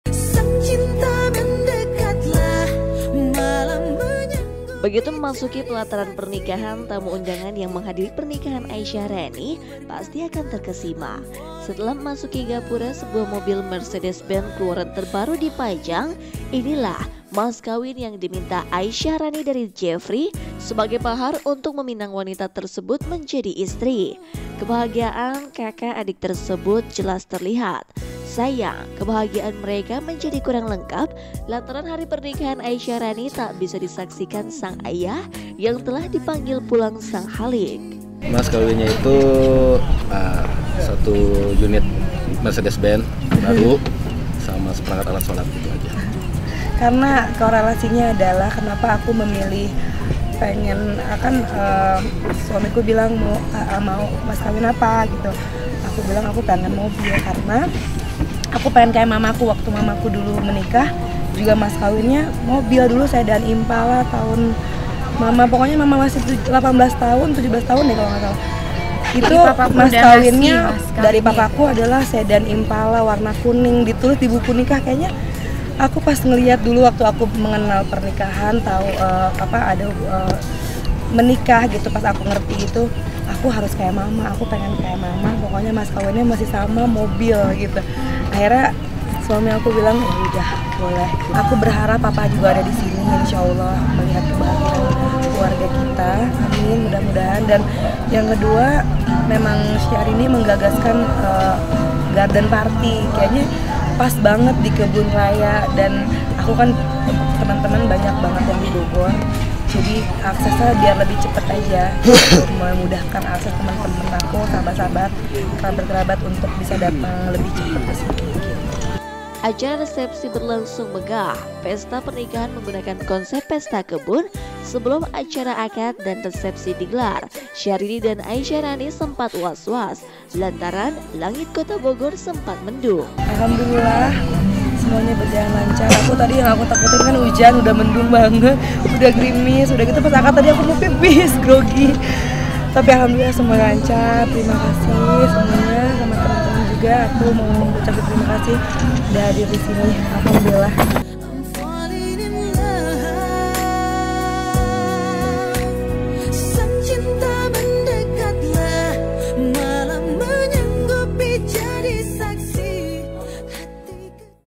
Ups.. Adik Menikah, Syahrini Iri?
Kabar bahagia datang Aisyahrani, adik sekaligus manajer dari Syahrini. Aisyarani melangsungkan pernikahan dengan kekasihnya Jeffri, Sabtu (5/6), di Kebun Raya Bogor. Jika kebanyakan pasangan menikah memilih emas sebagai mas kawin, tapi tidak dengan Aisyahrani. Ia meminta mas kawinnya berupa mobil Mercedez Benz keluaran terbaru. Benarkah hal itu membuat Syahrini iri? Mau tahu berita selengkapnya? Simak videonya hanya di KapanLagi!